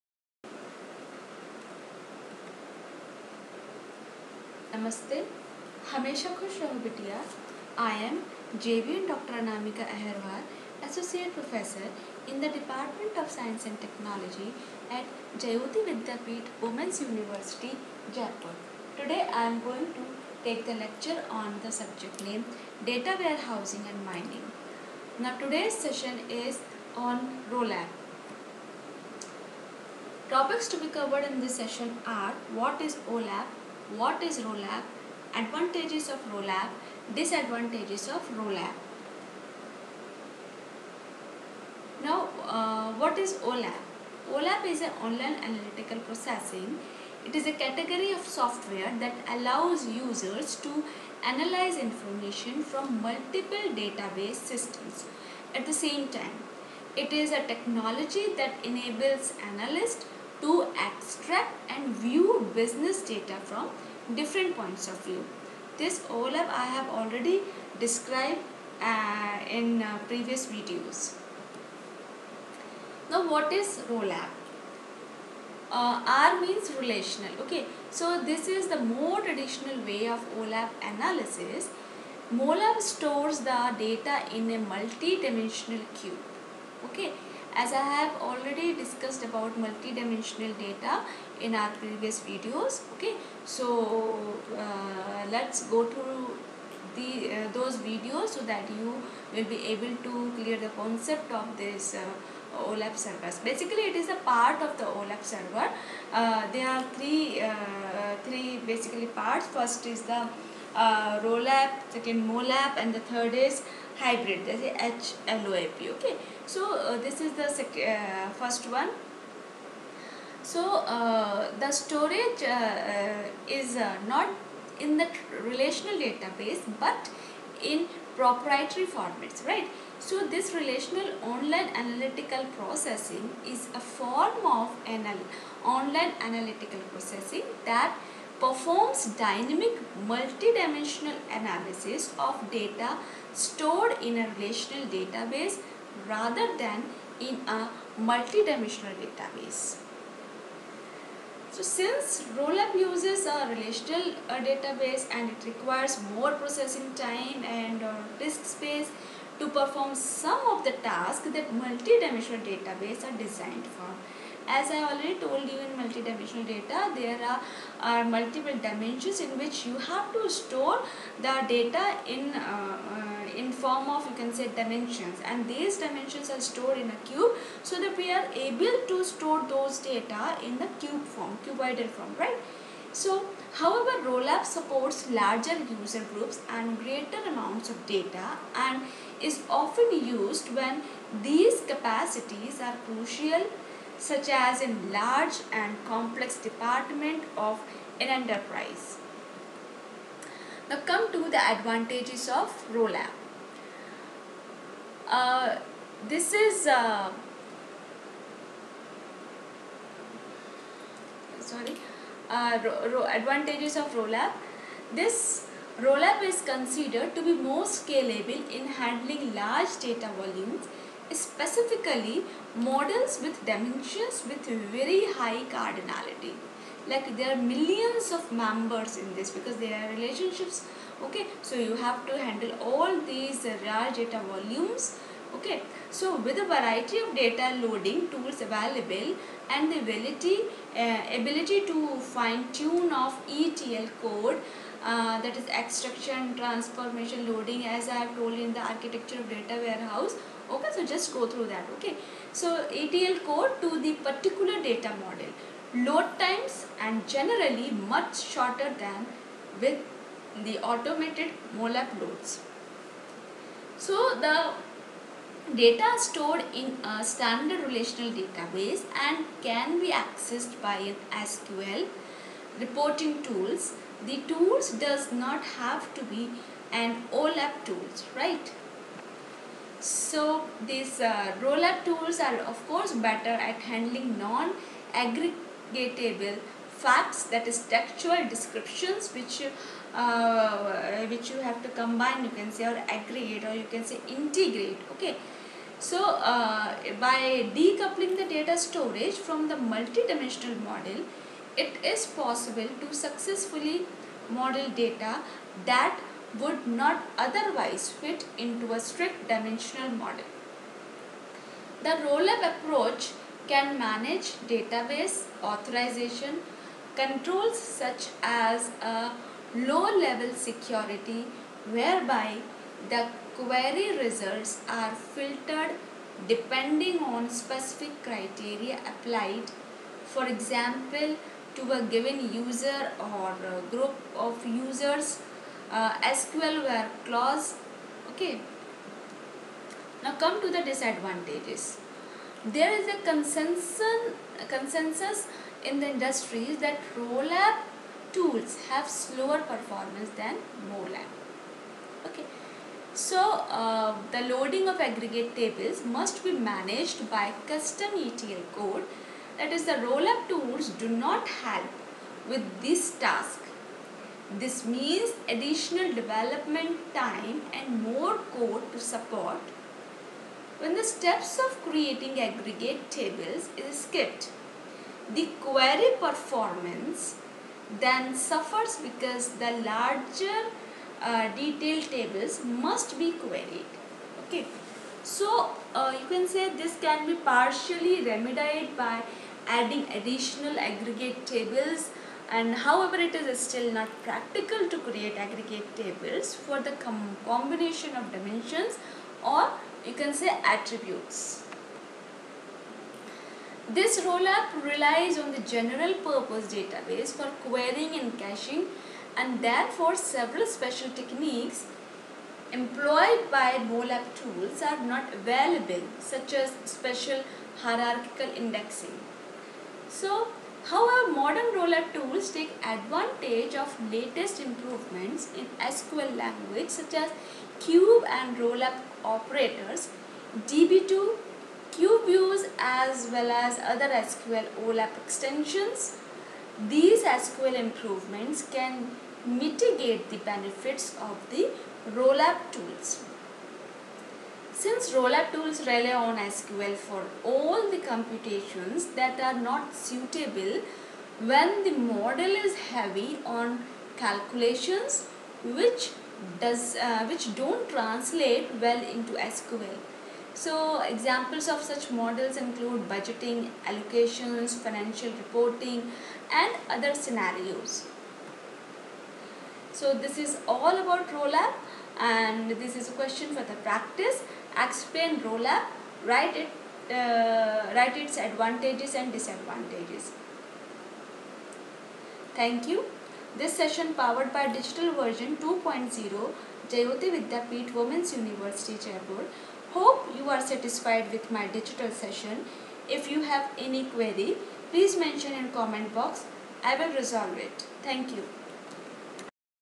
Jayoti Vidyapeeth - ROLAP
Jayoti Vidyapeeth - ROLAP. Name-JVn Dr.Anamika Ahirwar Dept. of Science & Technology, Under Faculty of Education & Methodology Course Name- Data Warehousing & Data Mining. Session Name- ROLAP. Class- DET + B.Tech CS trimester 9.